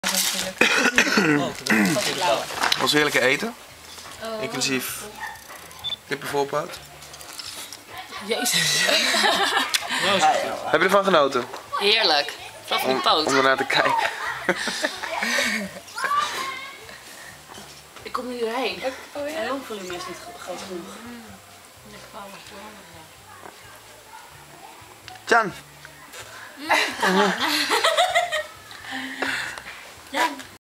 Soep eten bij Chon en familie in Laos
Soep bij Chon: Kippensoep voor sterke maag